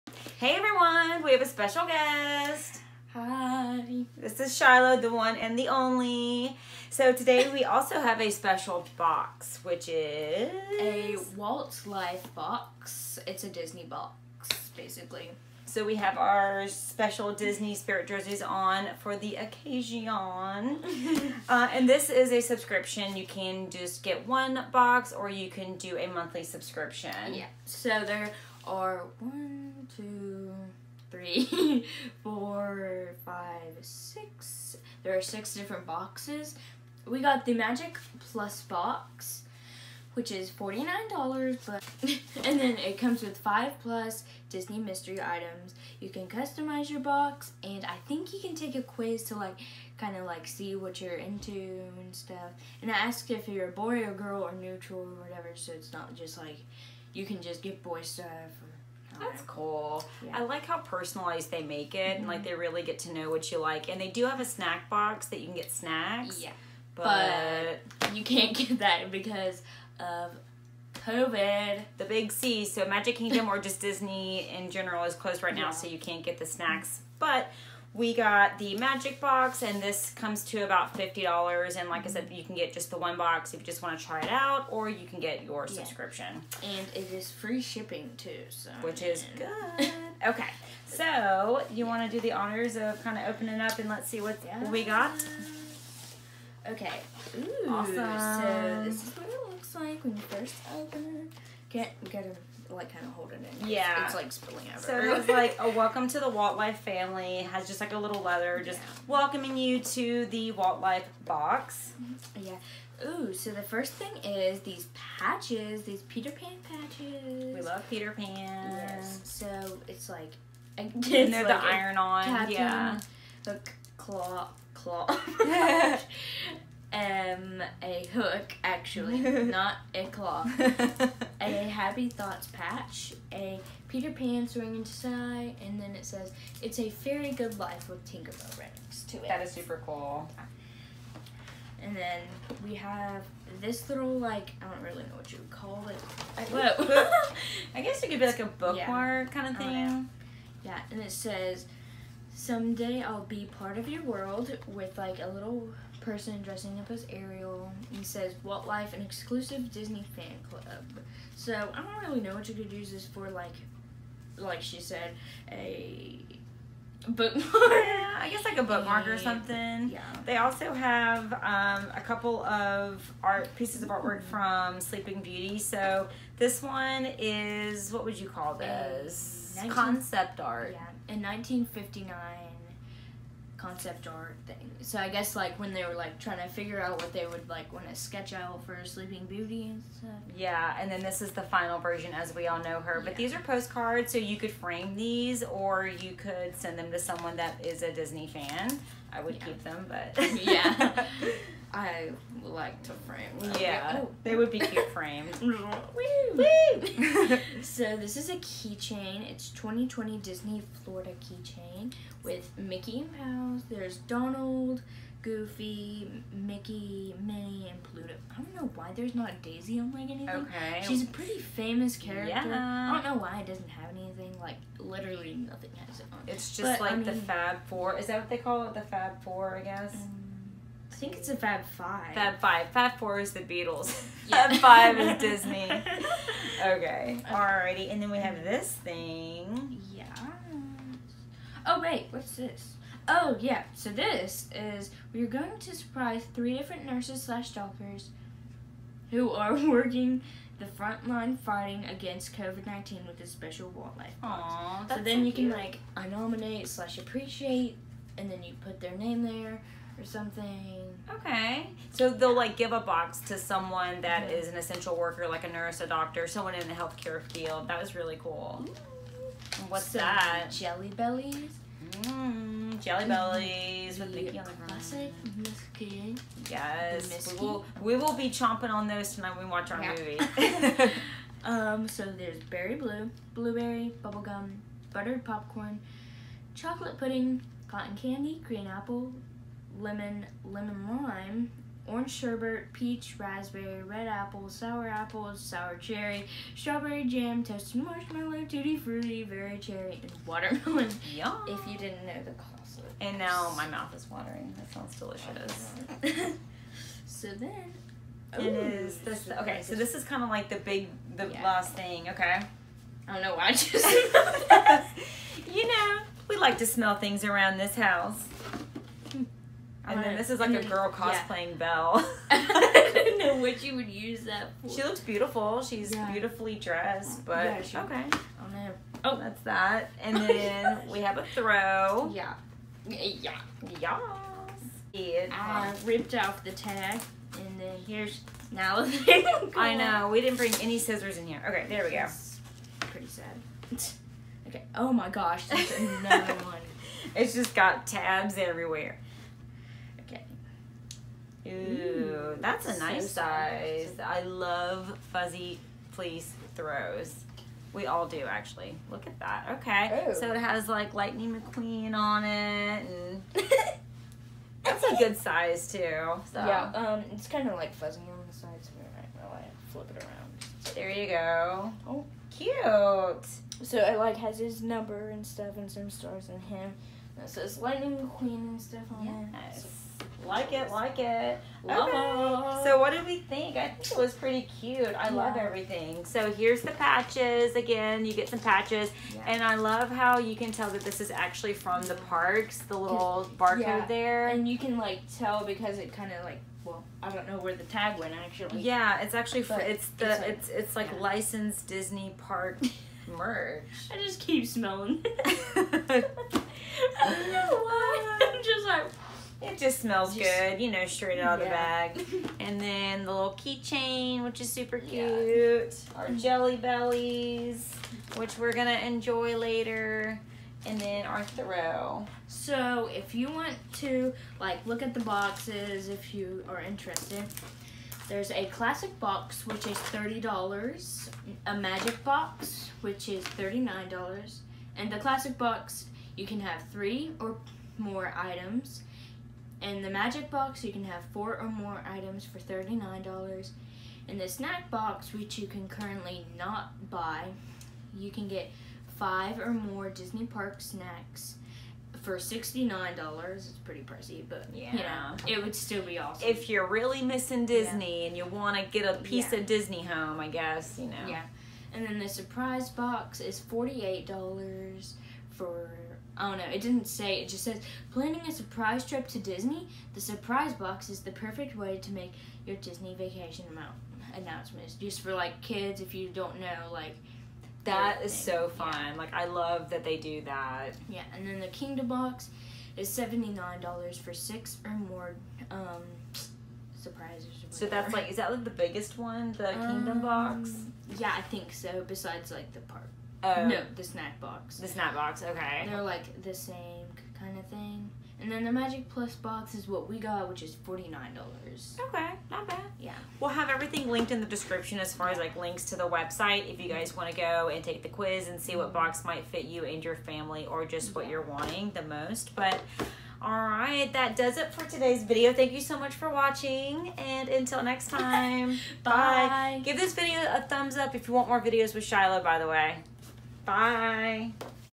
Walt Life Disney Box
Hey everyone! Today, Shiloh and I are unboxing Walt Life a Disney subscription box. We are major Disney people are we are missing our “Happy Place”!

Instagram.com/WaltLifeBoxes